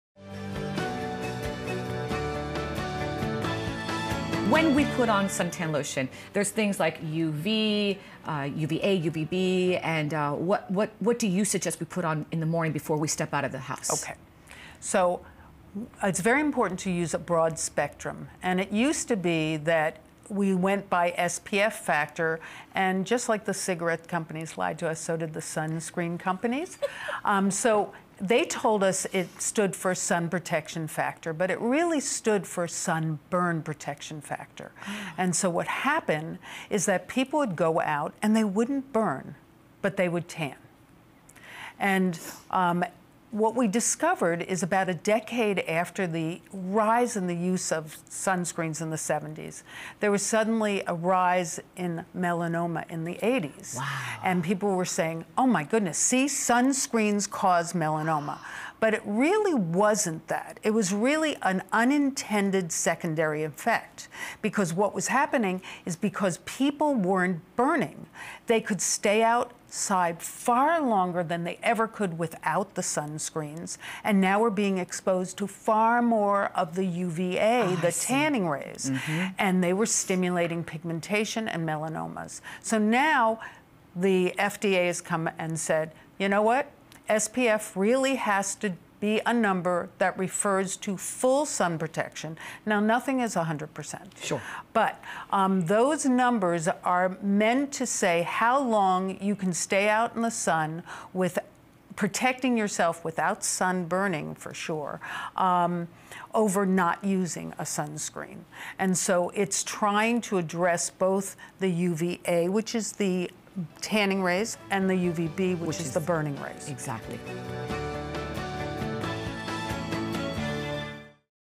Skin Aging and Sunscreen
Dr. Judith Crowell, Dermatologist with Baptist Health South Florida, explains SPF factor was not for sun protection factor, but for sunburn protection factor. People were not burning, but they were stimulating pigmentation and melanomas.